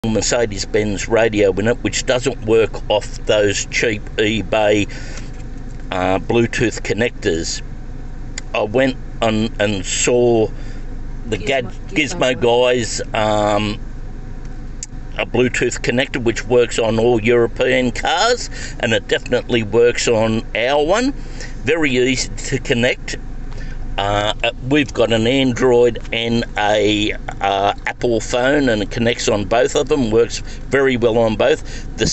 Mercedes SL Class R129 Radio Upgrade for Bluetooth Music Thru Original Factory Radio
for Bluetooth Music Thru Original Factory Radio

Bring new life to your Mercedes SL Class R129 Radio Upgrade with the latest technology! With Gizmo Guy Gadget, you can easily add Bluetooth capabilities and stream music, podcasts, audiobooks, and more from your phone through your Mercedes SL Class R129 original sound and Navigational system. No need to replace your head unit and sacrifice the integrity or resale value of your car, simply upgrade and modernize your sound and infotainment system without any modifications.